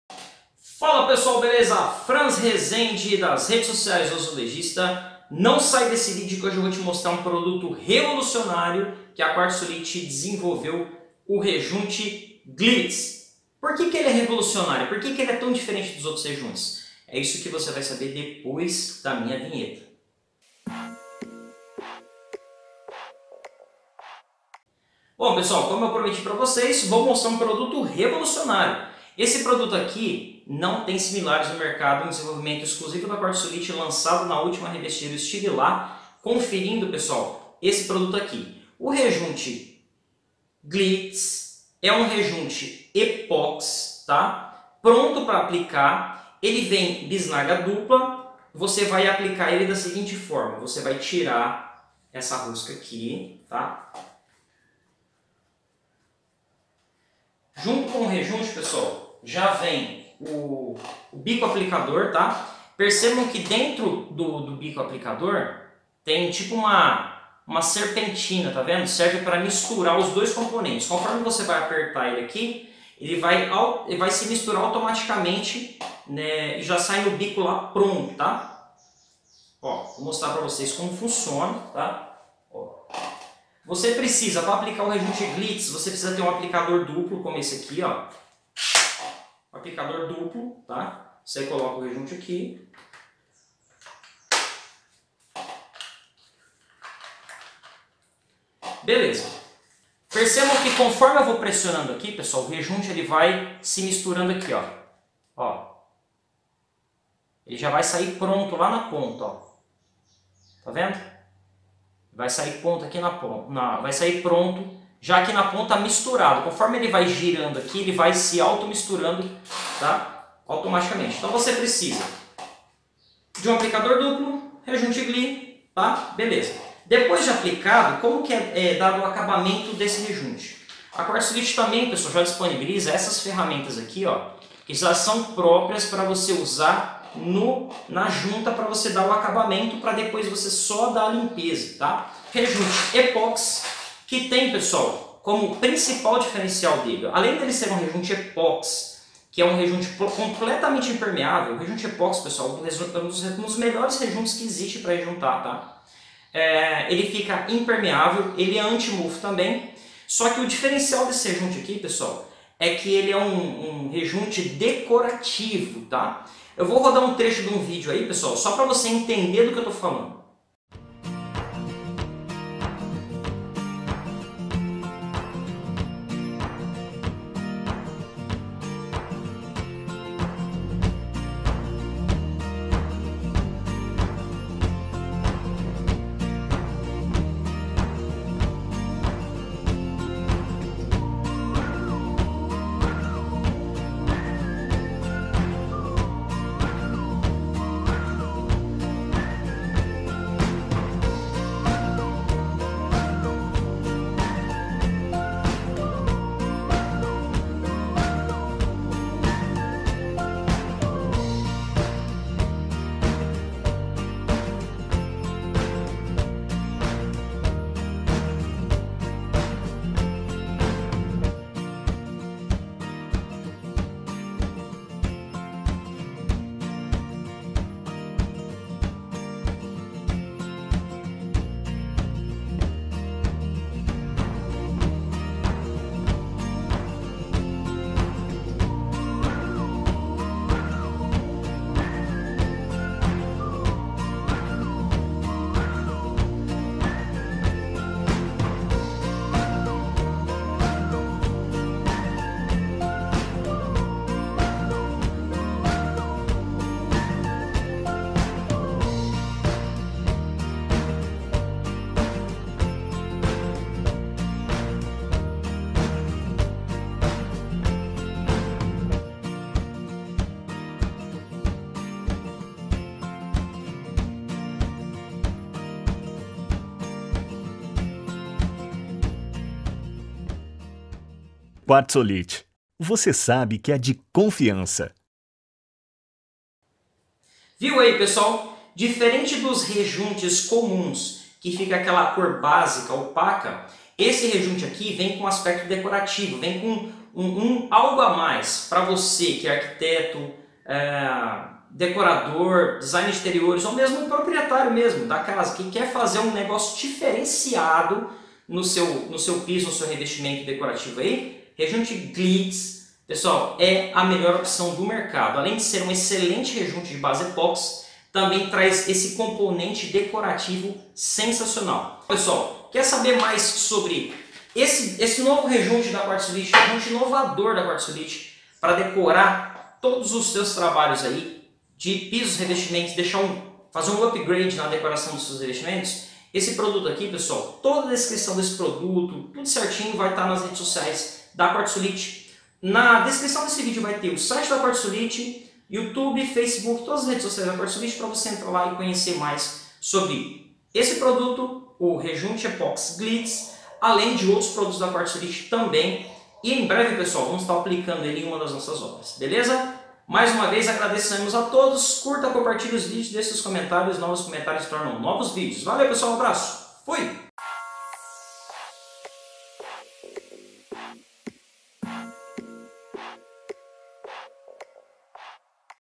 Rejunte epoxi para porcelanato diferente dos outros
Quer ficar por dentro das novidades do O Azulejista?

Neste vídeo vou te mostrar o rejunte mais revolucionario que eu já vi.
O Rejunte Glitz é um rejunte de base epóxi;
Dispensa a mistura manual, basta utilizar a pistola dupla quartzolit e acoplar o bico misturador ao produto;
São 4 cores especiais(ouro,prata,cristal e cinza cromado)para dar aquele destaque no seu piso ou revestimento
Secagem "destaca fácil" - aguarde 6h e destaque a sujeira grudada no revestimento.
Para saber mais sobre este incrivel lançamento acesse o site da Quartzolit no link abaixo: